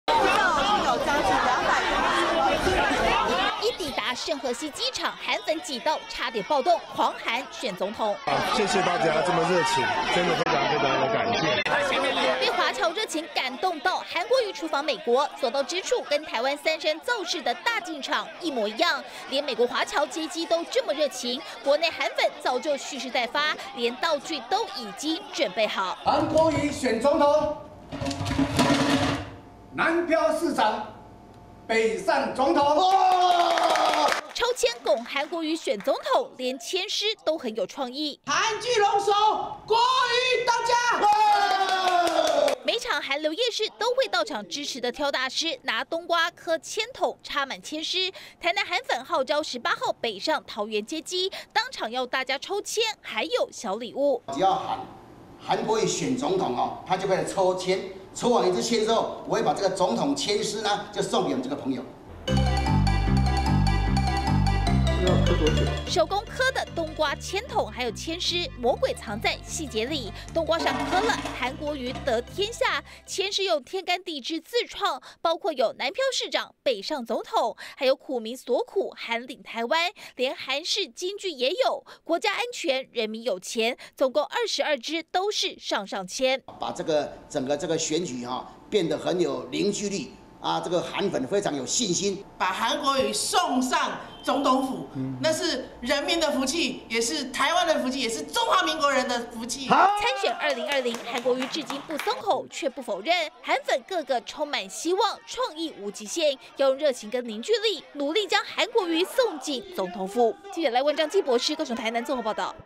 201904016中天新聞 全台韓粉發動接機！ DIY籤筒「韓國瑜得天下」
高雄市長韓國瑜訪出訪美國，所到之處擠滿韓粉，狂拱「選總統救台灣」，在聖荷西機場差點擠到一片混亂，不只華僑熱情，國內韓粉早就蓄勢待發，不只高雄，現在連台南都有韓粉要發動接機，還有每次韓流夜市都會出現的虱目魚挑大師，這次用冬瓜刻了一個籤筒，自創了22支上上籤，都是要拱韓國瑜選總統，當天只要有韓粉喊出通關密語，就可以抽籤、再送一張拱韓選總統貼紙，要將呼聲衝到最高點。